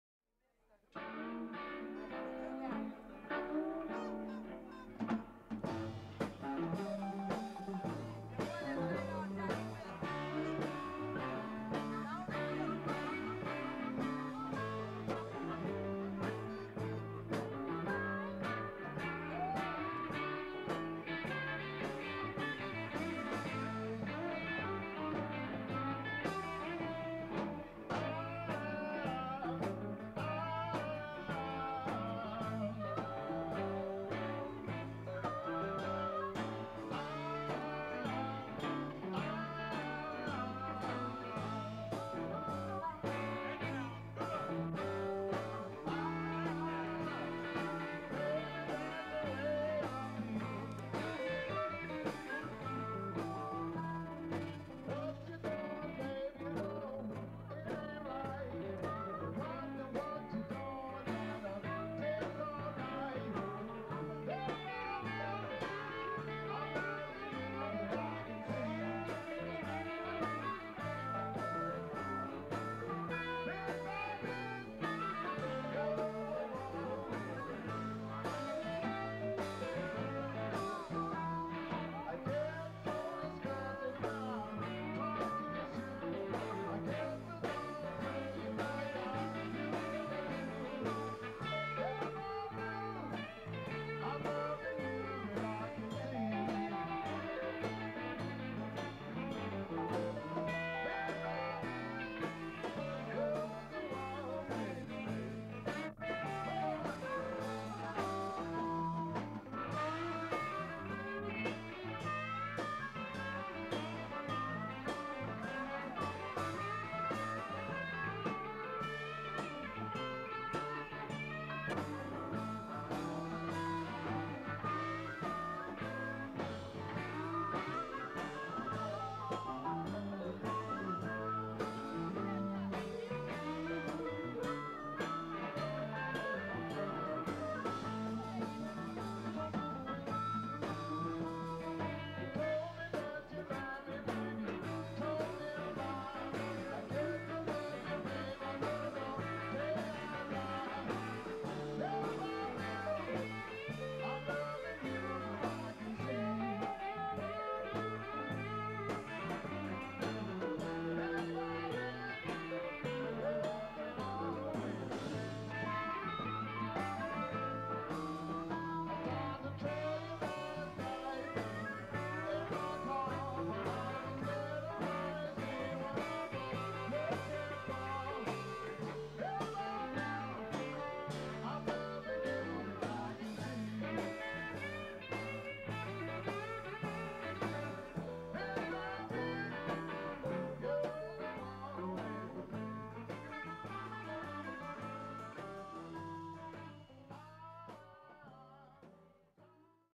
Chicago bluesman Willie Buck c.1990
Willie Buck has been on the Chicago blues scene since the 1950s, but just released his first CD (a reissue of the vinyl LP he released in the early '80s) on Delmark Records.  This clip is from a New Years Eve show at Roberts 500 Room on Chicago's south side, with an all star band featruring Louis and Dave Myers of the Aces, Moose Walker, and Little Mac Simmons - almost the same line up as his CD, but with a different drummer.  Willie continues to play in the Chicago area.